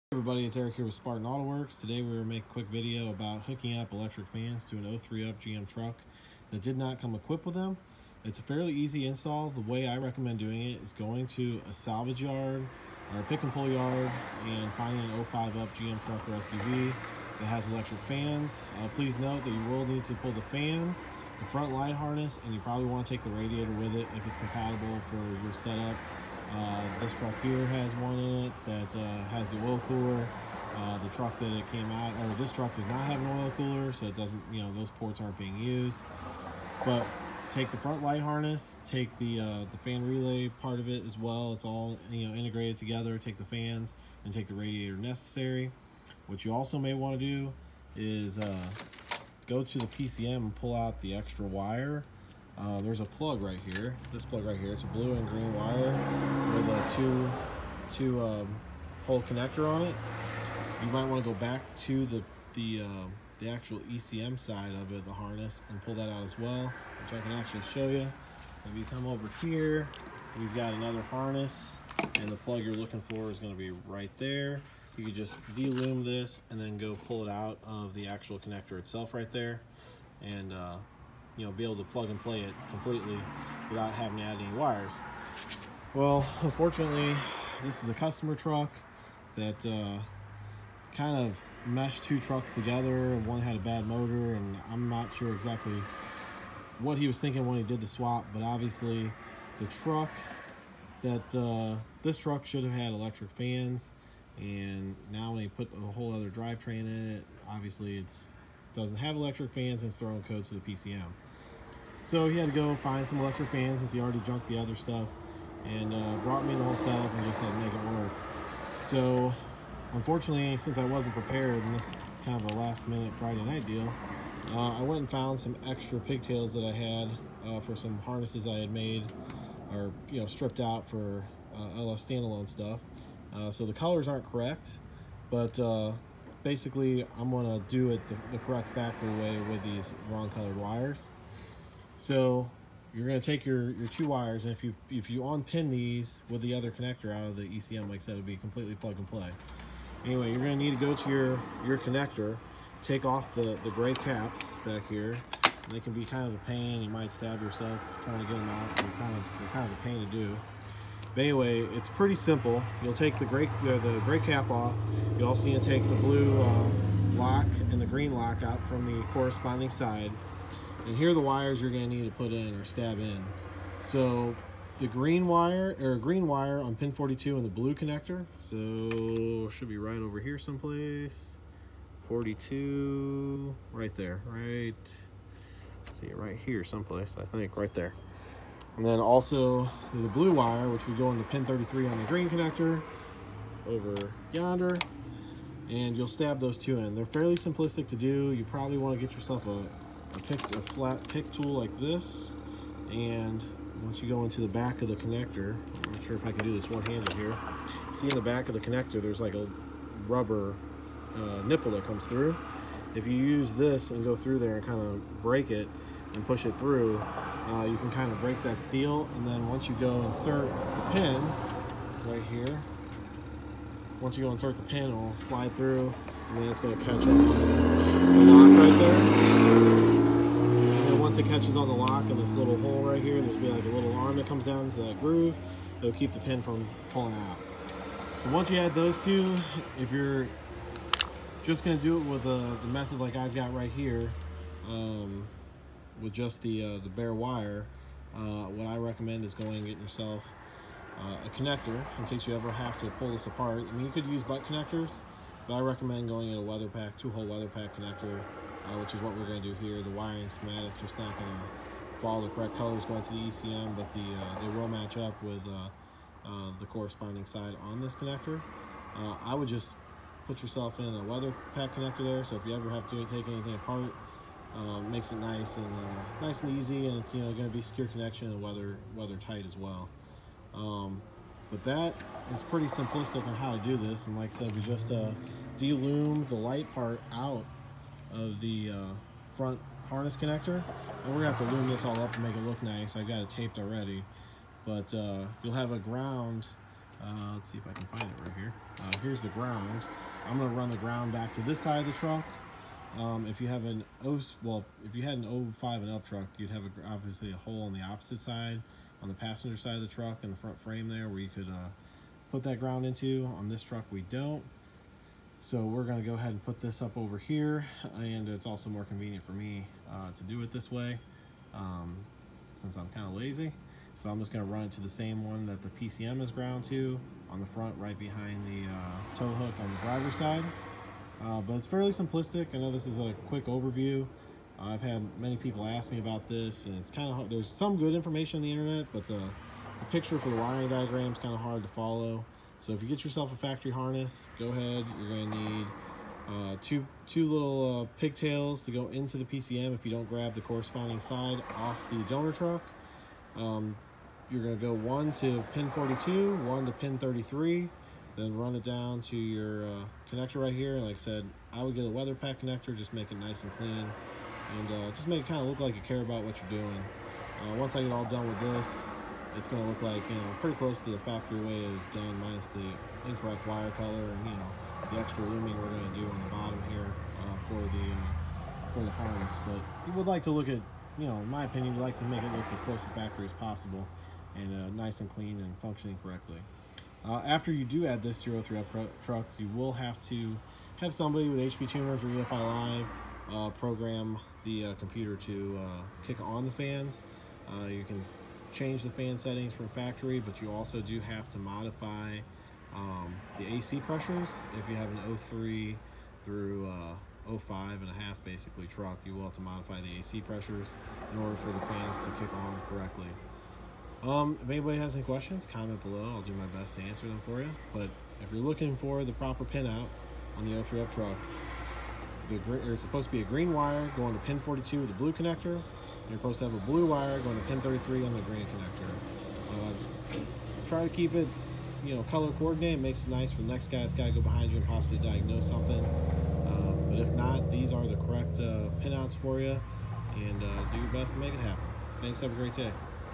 Fan wiring Install on GMT-800 Trucks and SUVs. Plug and Play
Electric Fan PCM connection on GMT-800 Trucks. This shows how to connect Fan trigger wires to the Blue and Green PCM connectors on 03-05 3/4 and 1/2 ton trucks. This will also work on 3/4 ton trucks 2005 and newer. All 2005 and newer 1/2 ton trucks came factory equipped with electric fans.